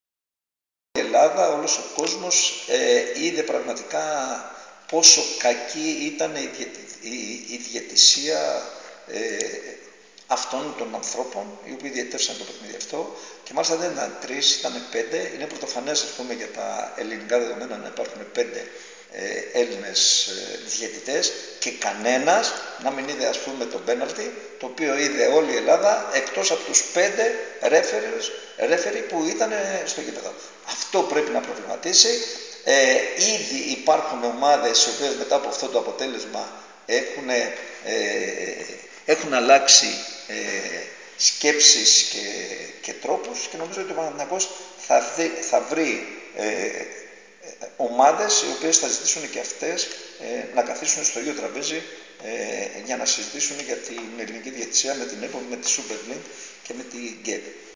Βασίλης Κωνσταντίνου στο paoworld 5
O Βασίλης Κωνσταντίνου μιλά στο paoworld για τον Παναθηναϊκό του σήμερα